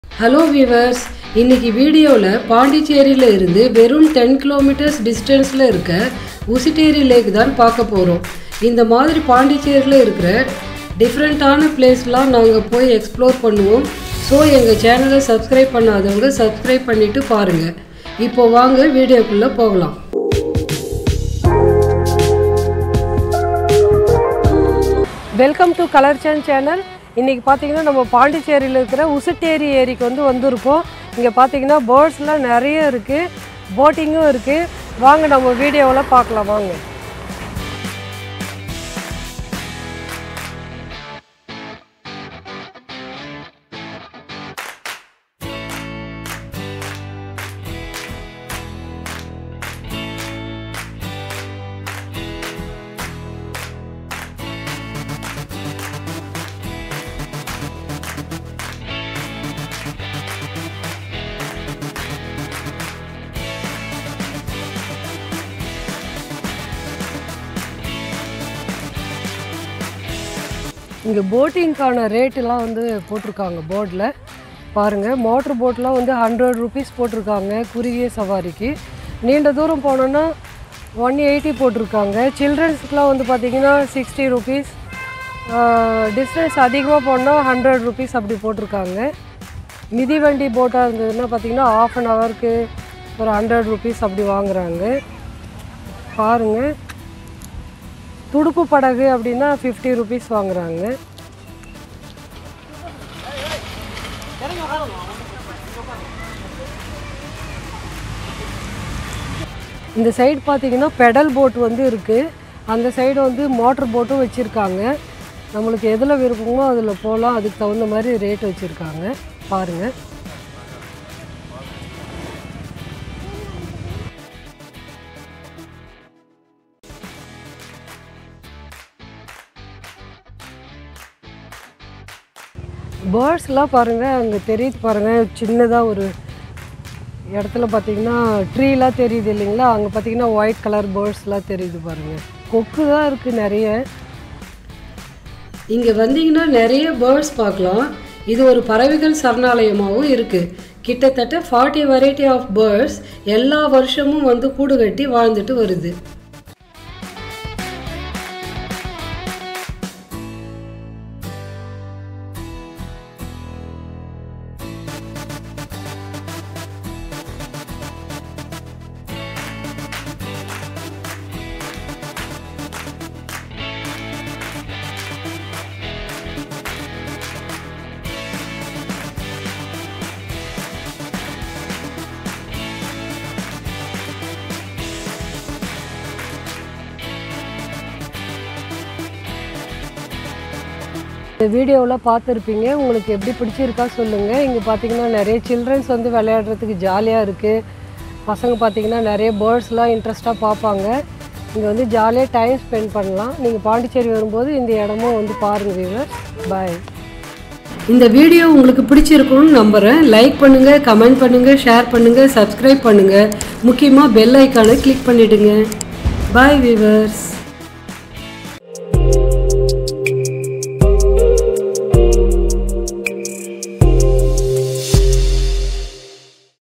Pondicherry Ousteri Lake | Boating | Bird Sanctuary | Nature Trails | Kids Play | Tourist places
Ousteri Lake, also known as Osudu Lake or Osudu Pallam, is a stunning freshwater lake located in the Union Territory of Pondicherry

The lake spans an area of approximately 390 hectares and serves as a vital source of drinking water for the local population. Experience the vast expanse of sparkling blue water and lush greenery that surrounds the lake.

It is a gorgeous place to relax and spend some quiet moments with your loved ones on holiday; it is a sight to behold. Puducherry is a true blue vacationer's paradise as the time comes to a standstill while chilling and distancing your mind from the chaos.

Things to do